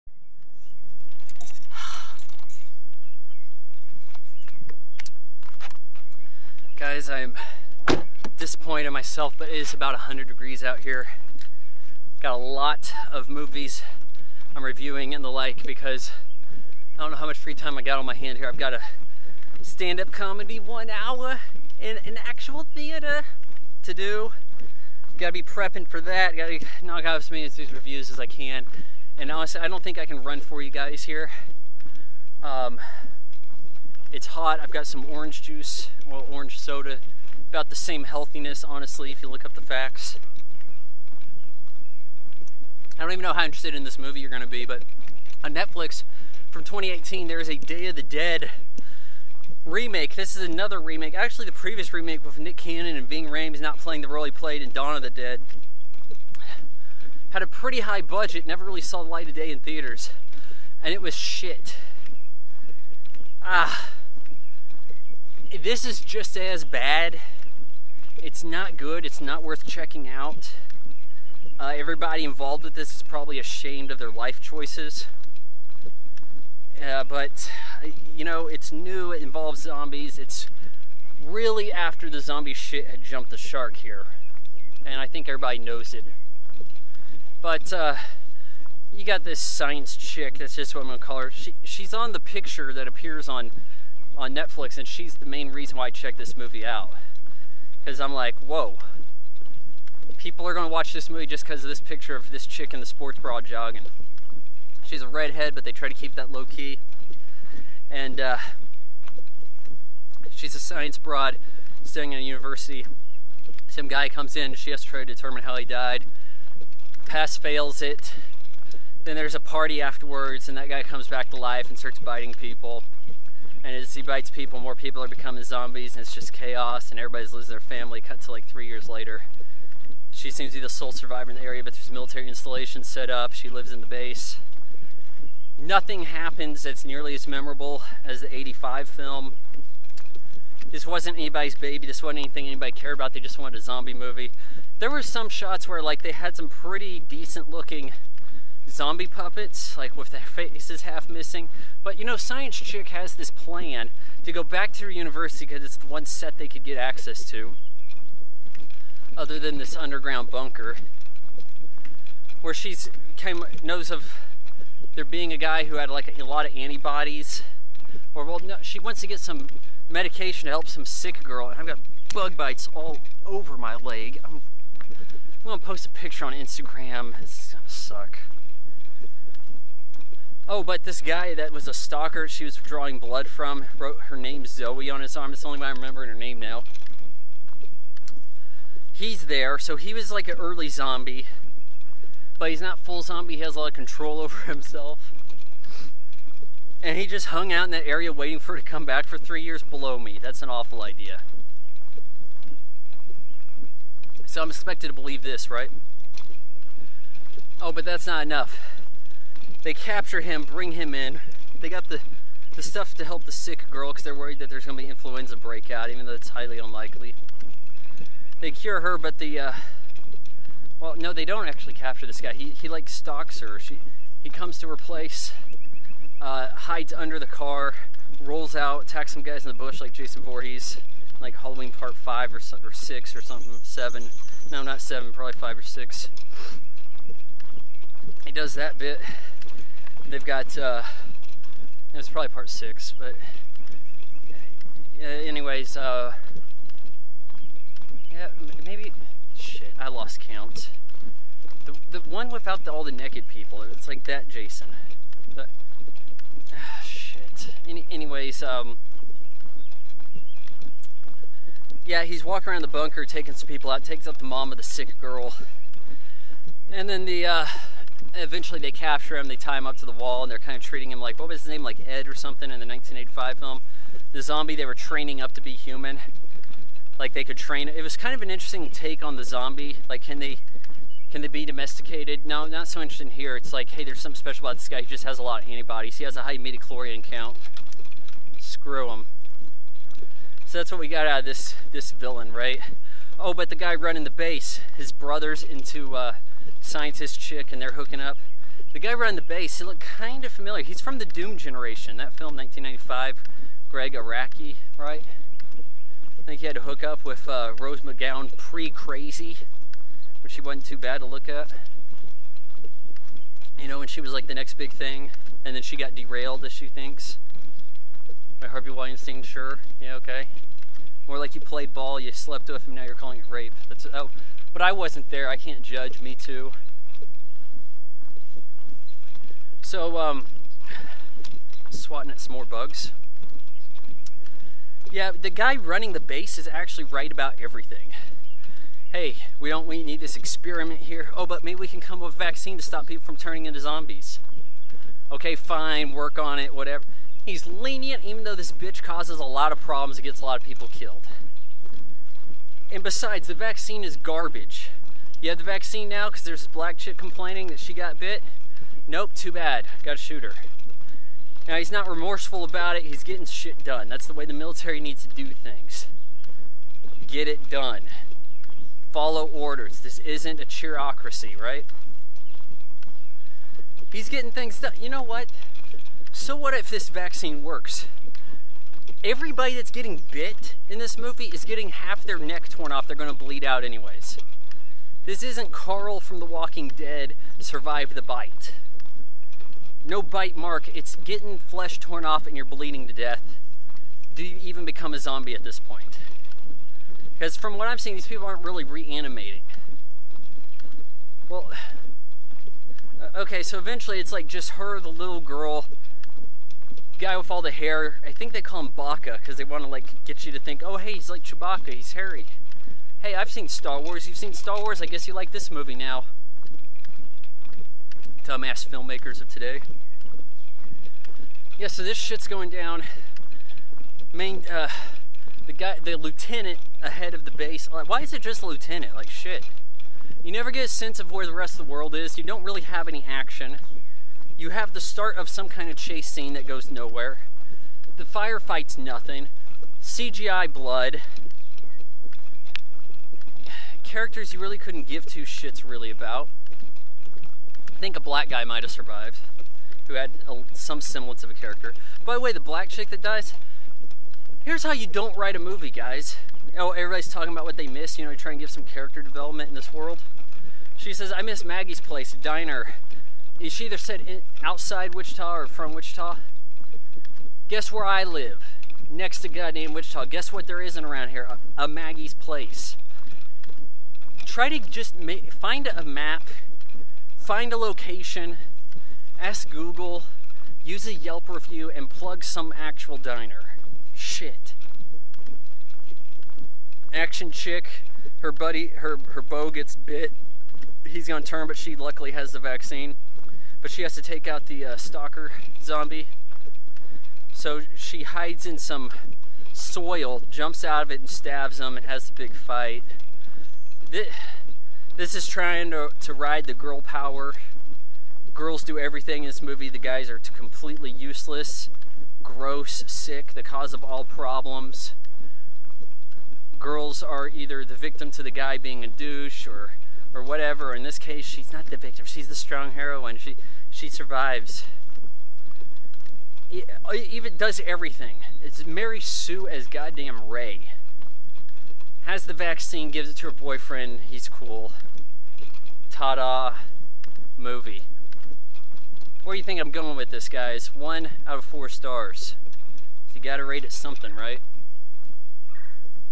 Day of the Dead: Bloodline SUCKS - Movie Review / Rant
Day of the Dead: Bloodline Sucks.

Day of the Dead: Bloodline is a 2018 Bulgarian-American action horror film directed by Hèctor Hernández Vicens and the screenplay by Mark Tonderai and Lars Jacobson based on the characters created by George A. Romero. The film stars Johnathon Schaech, Sophie Skelton, Marcus Vanco, and Jeff Gum. It is one of two remakes of Romero's original 1985 film Day of the Dead, with the first released in 2008. The film was released on January 5, 2018.